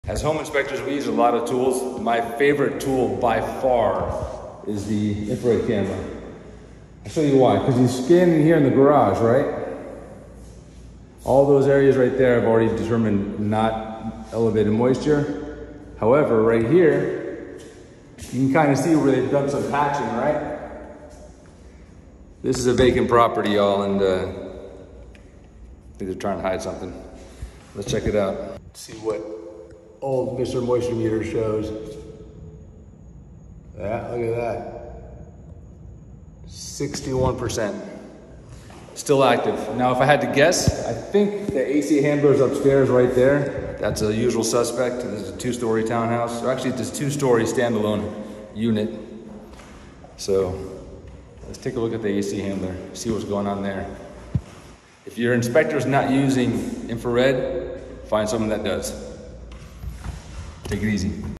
Infrared leak detection, a must on EVERY inspection!
infrared leak detection is the most effective way to locate water intrusion. I will not inspect a home without using infrared technology. Not only can it detect leaks, but it is excellent in electrical overheating detection as well! Always make sure your inspector uses infrared technology. It is a GAME CHANGER!! 💪😎

Remember, the only thing more expensive than hiring a professional is hiring an amateur! Be sure to hire well qualified, properly insured inspectors and contractors.

Happy Hunting!

Toby